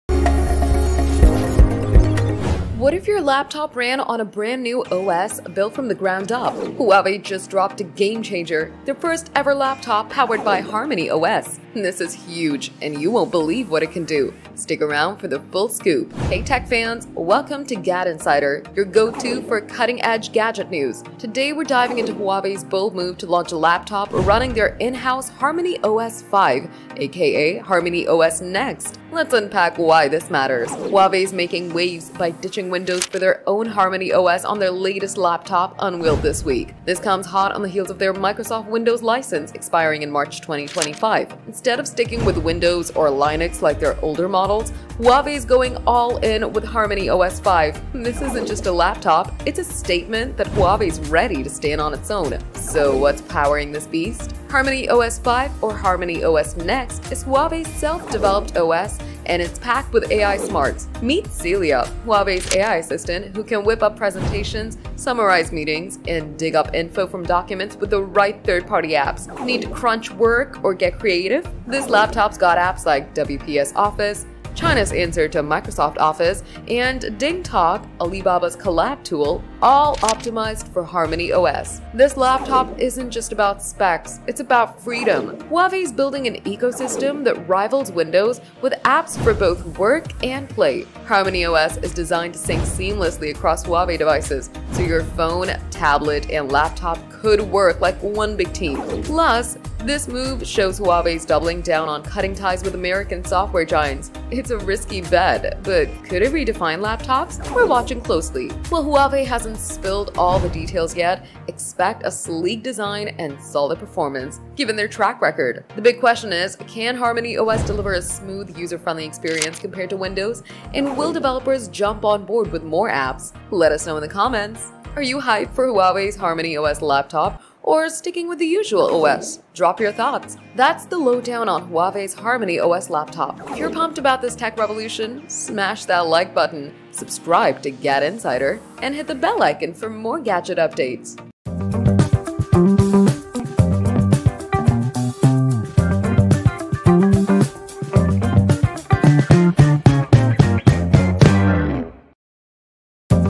Huawei Just Launched Its OWN Laptop OS – Microsoft Should Be Worried 😳
Huawei is officially entering the next tech era with the launch of its first-ever laptop powered by HarmonyOS, ditching Windows and taking full control of its software ecosystem! This bold move could reshape the future of computing, especially in China. In this video, we explore the laptop’s features, what HarmonyOS on PC really means, and how it could challenge Microsoft and Apple in the global market!